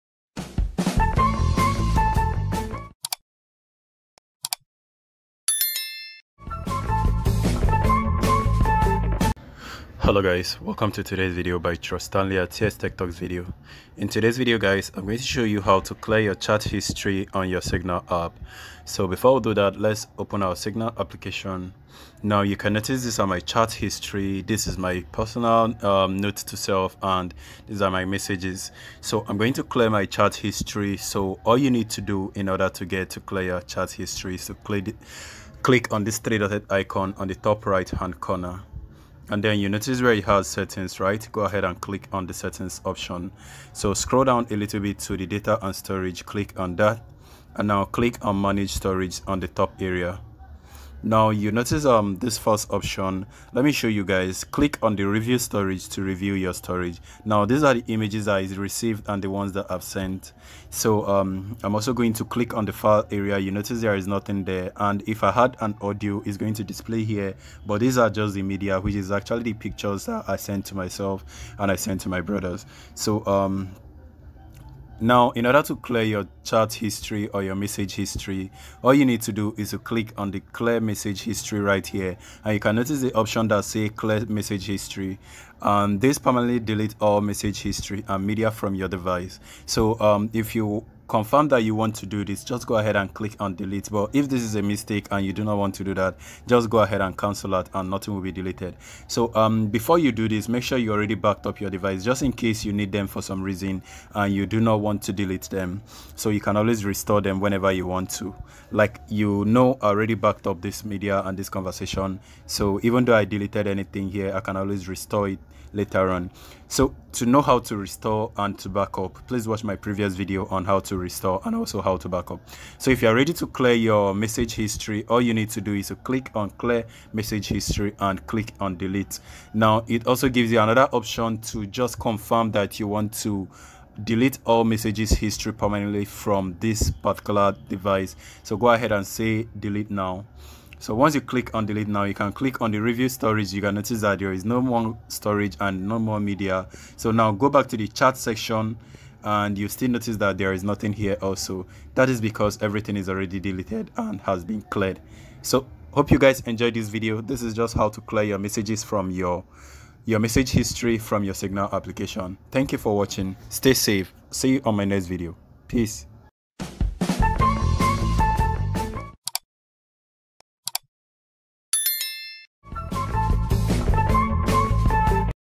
How to clear message history on Signal private messaging app | How to use Signal app
How to clear message history on Signal private messaging app, when you delete your Signal app message history it wipes all converstation on the signal app chat and you will not beable to see all your messages.

Note: do back up your chat before you do this in other for you not to loss any conversation by mistake and if you dont know how to do that do visit my channel Signal app playlist to watch how to backup and restor converstaion on signal app.